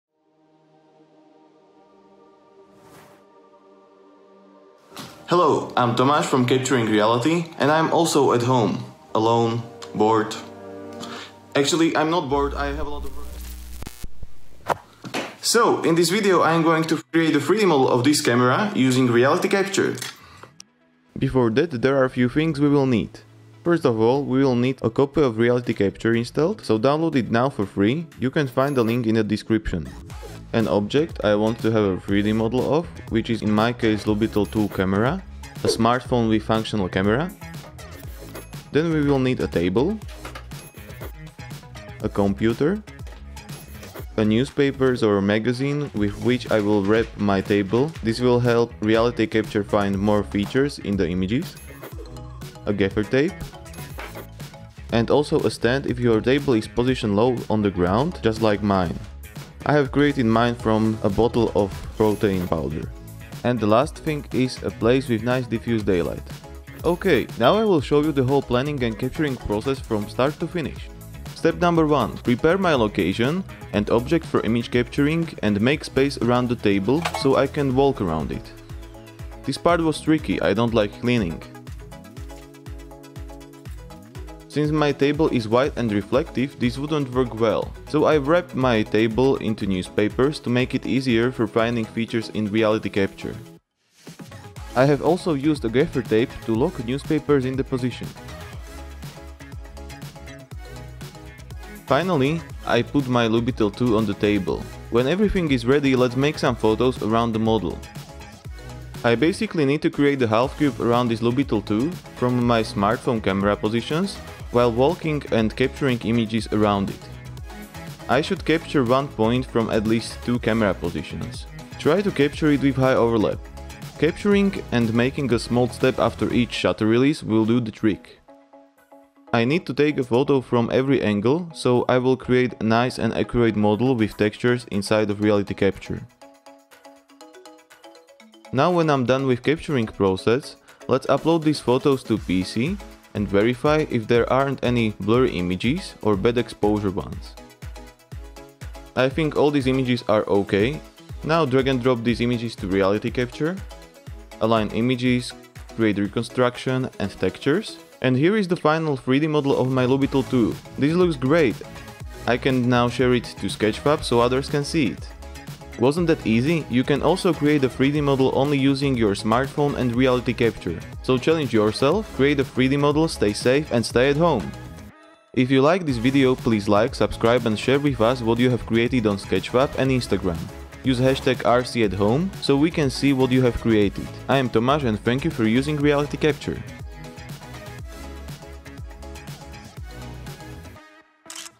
RealityCapture tutorial: 3D Scan With Your Smartphone
In this tutorial, we will show you how you can create a 3D model simply using your camera phone. You don't need a fancy, expensive camera equipment to create 3D models.
With RealityCapture you can also 3D scan just using your mobile camera.
Now it's a great opportunity to learn new skills at home!
Share with us your 3D creations on Sketchfab / Instagram using RcAtHome.

00:20 Props used to capture the 3D model at home
01:25 Capturing the 3D model on a reflective surface
01:45 Taking photos with your mobile phone
02:22 Uploading and processing the model inside of RealityCapture

RealityCapture tutorial: How to create your first 3D model | Part 1